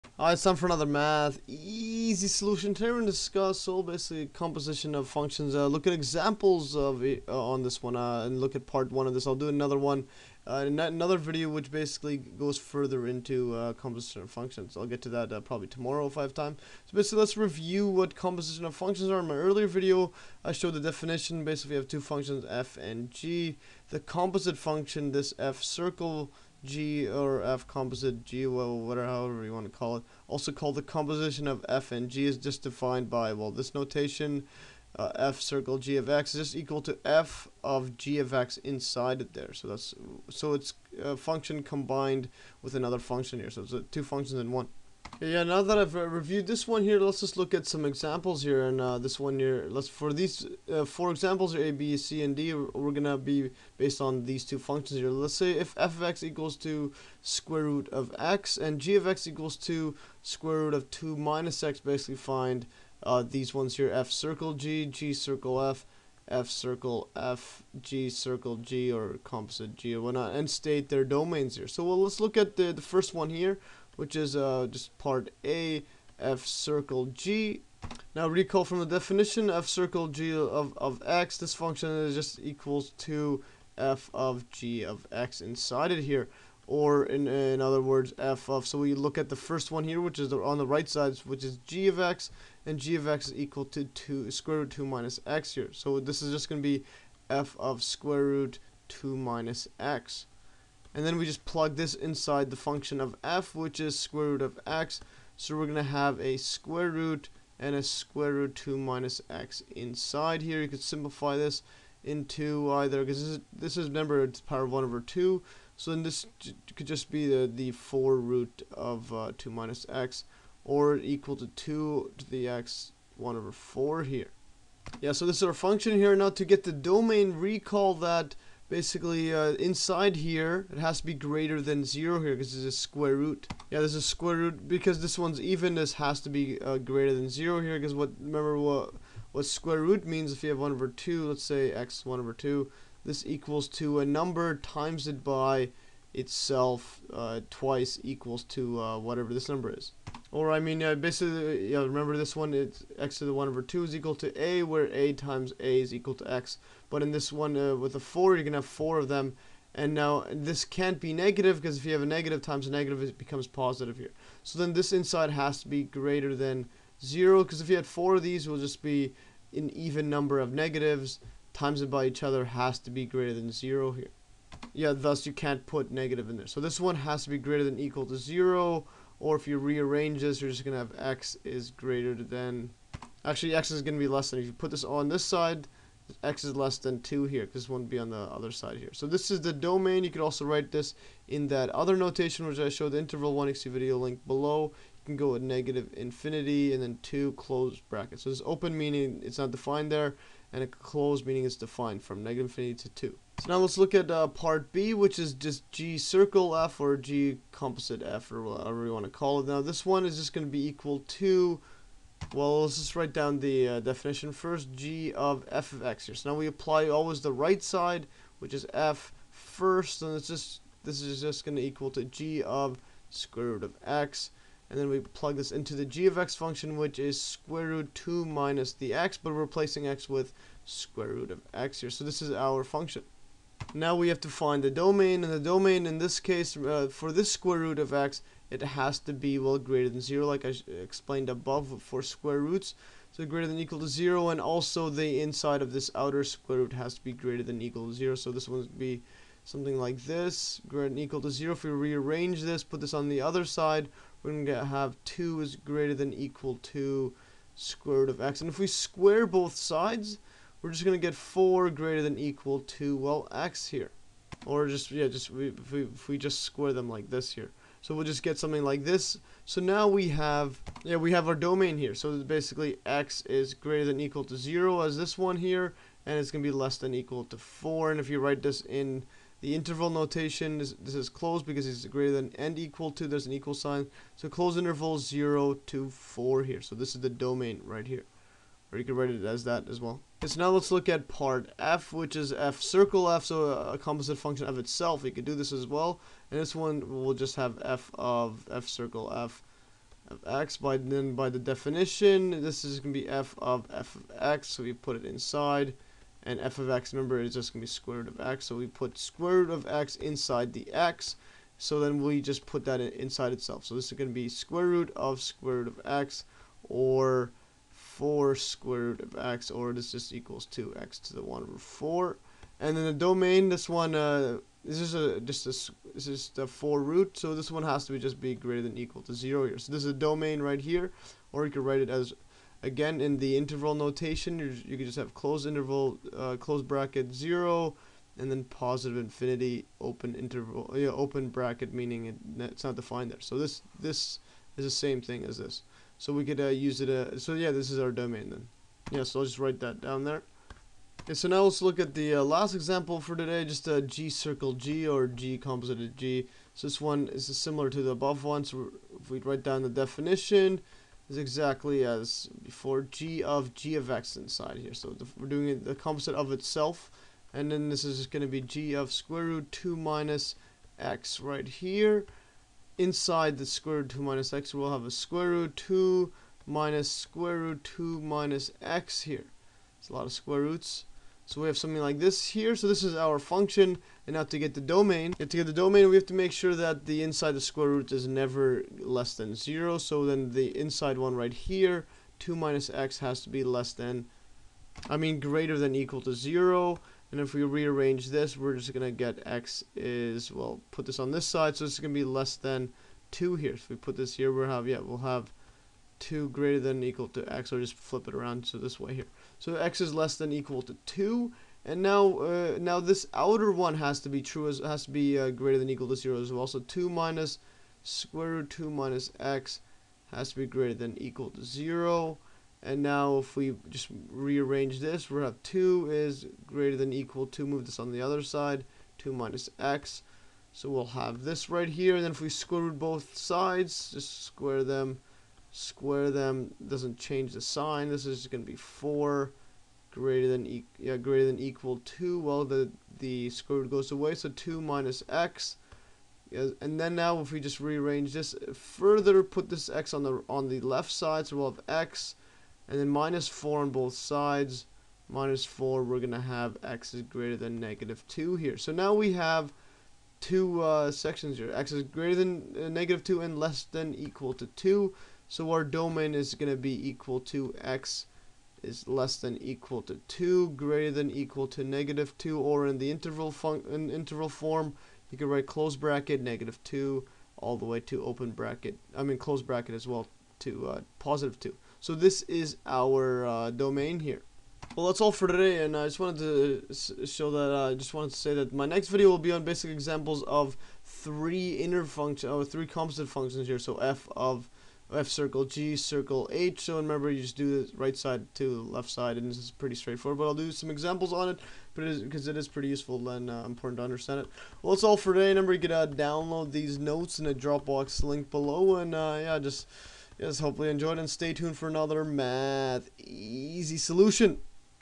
Composition of Functions - Examples Part 1
In this video I go further into composite functions and go over some very useful examples on determining the new composed functions as well as their corresponding domains.